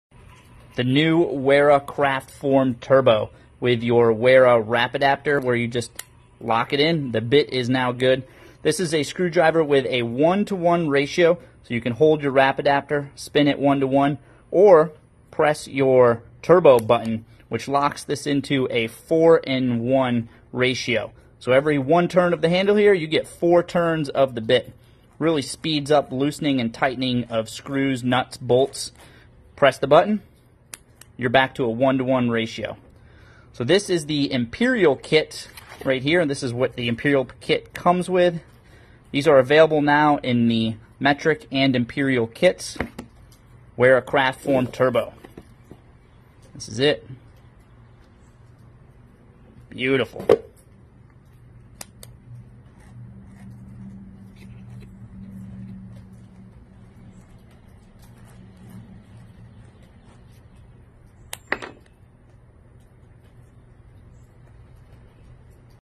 Wera Kraftform Turbo screwdriver review
Wera Kraftform Turbo screwdriver 4 to 1 ratio ‘turbo’ mode at the press of a button. Turbo mode is good for threaded fasteners that are already loose and need to be snugged up. Then switch to regular mode to tighten.